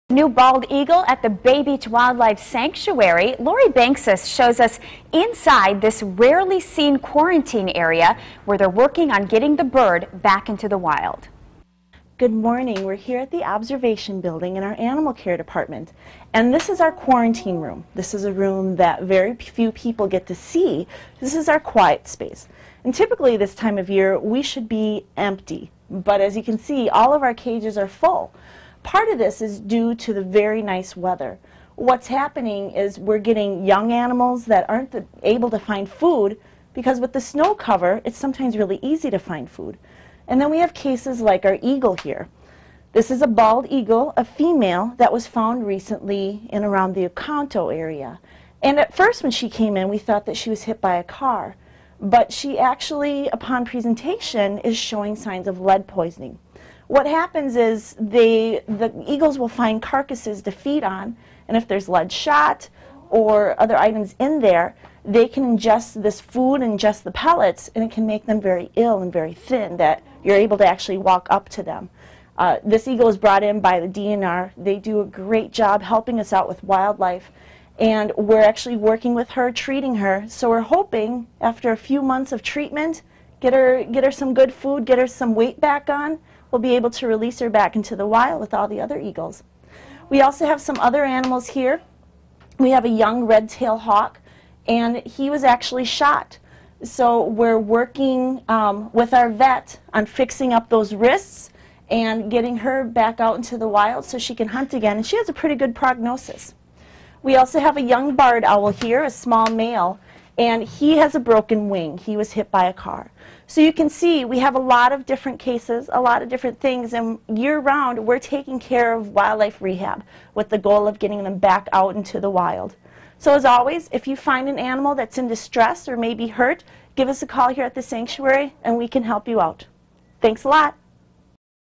GDW8SAT Wildlife Sanctuary
Lori Bankson shows us a bald eagle in treatment at the Wildlife Sanctuary.